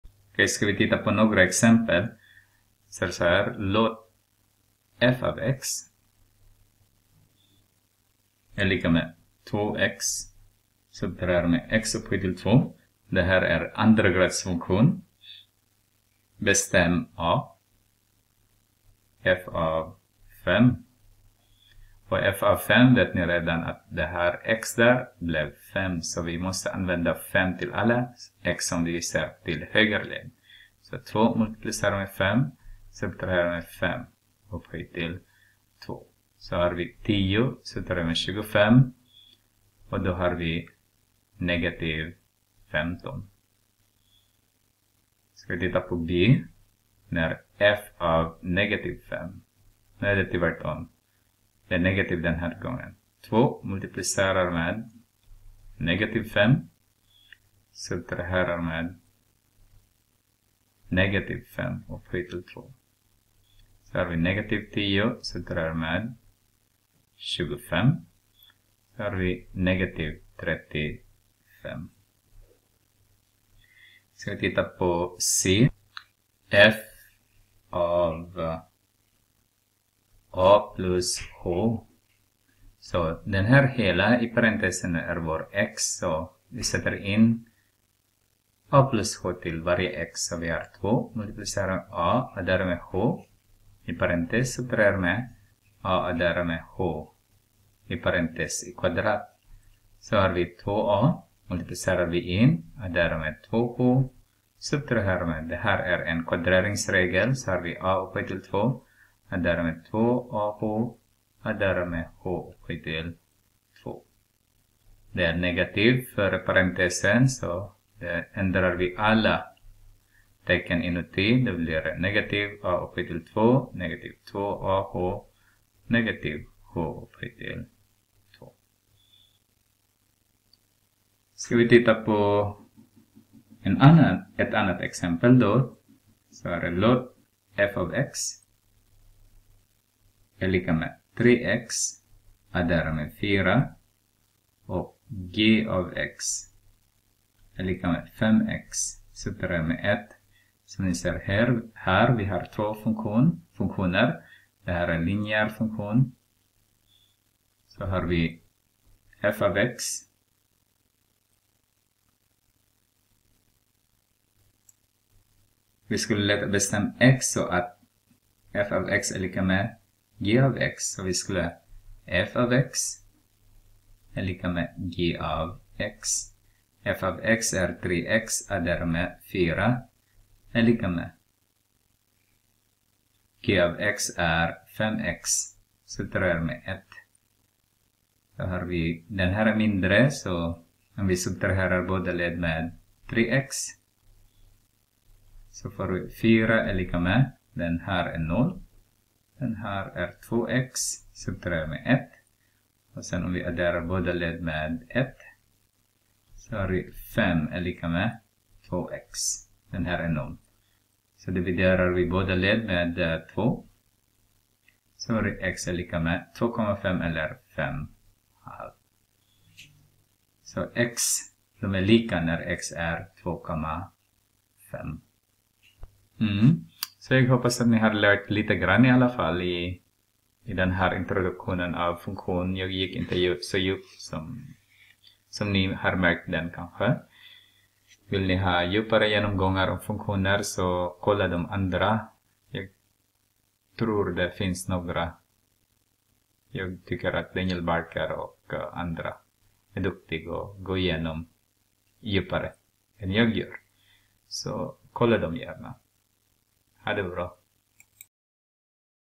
Genomgång av Matematik 5000 Ma 3b 3bc VUX Kapitel 1 Funktioner Inledning del 2
Genomgång av Matematik 5000 Ma 3b 3bc VUX   Kapitel 1   Funktioner   Inledning del 2       Besök gärna min google site för mer matte videoklipp :